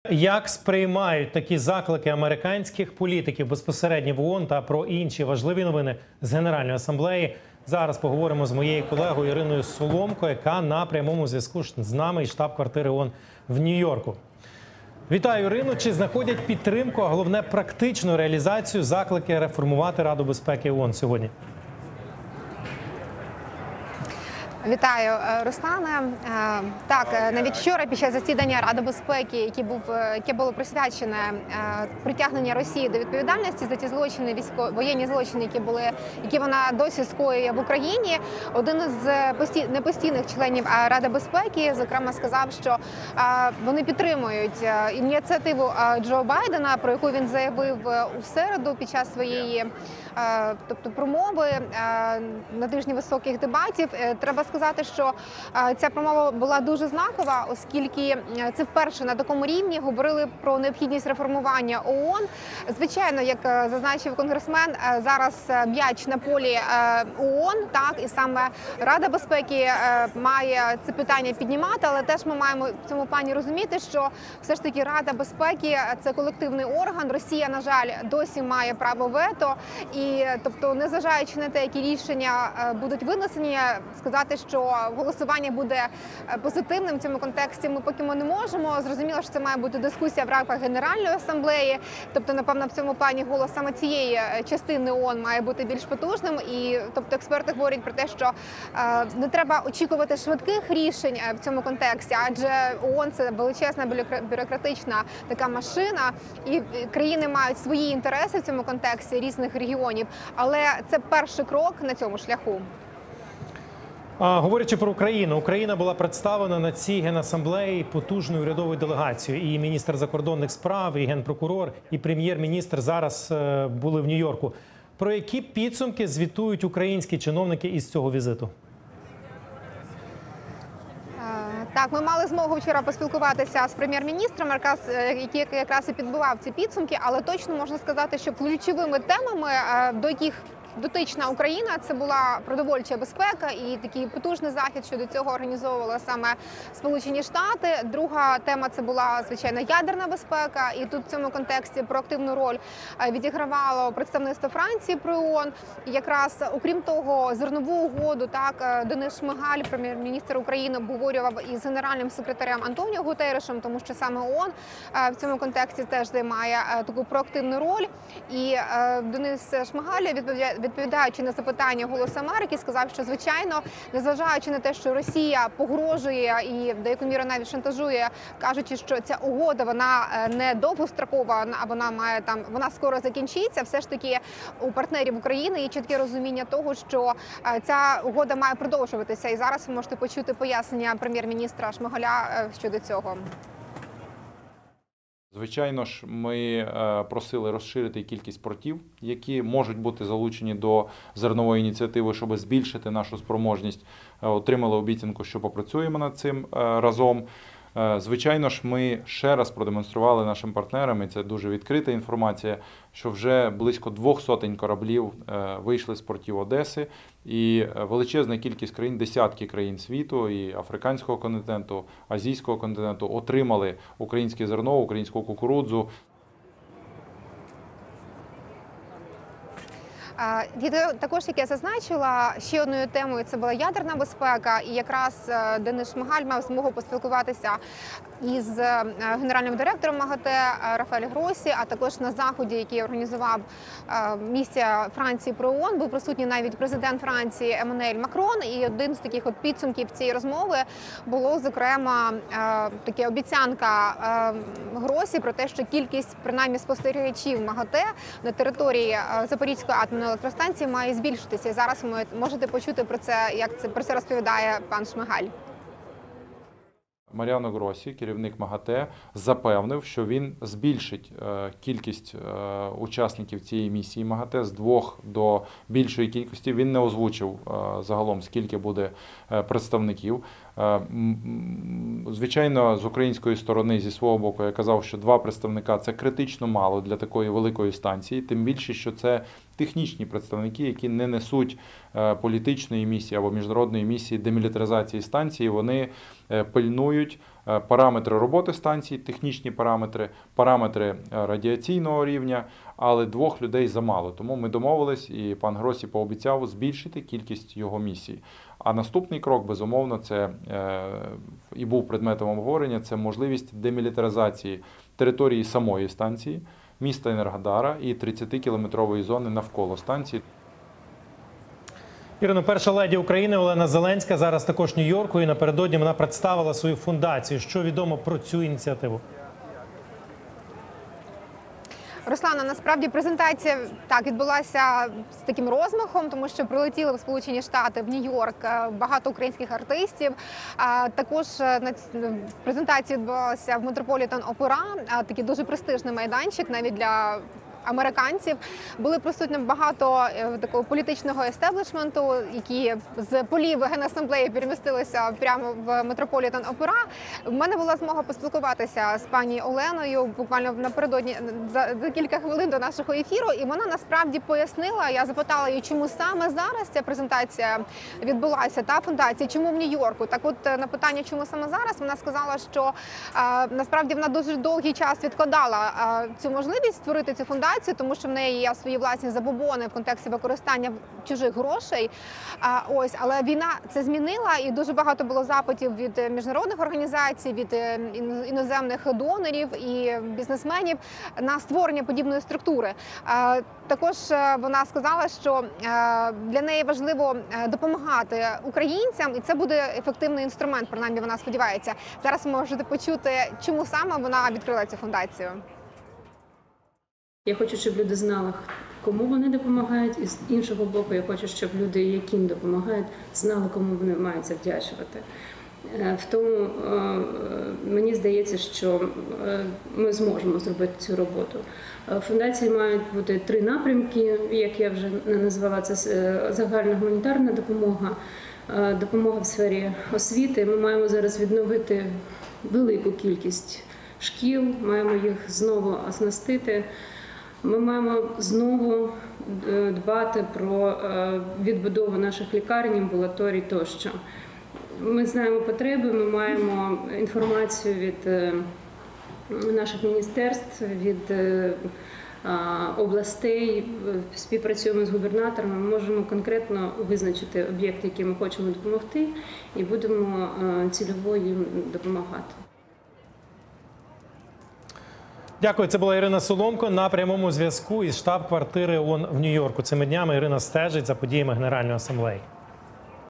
Ядерна та продовольча безпека – головні події Генеральної Асамблеї ООН для України
Що думають учасники Генасамблеї про виключення Росії з Радбезу ООН та про підсумки тижня роботи української делегації на цій щорічній події – розказує Ірина Соломко зі штаб-квартири ООН в Нью-Йорку.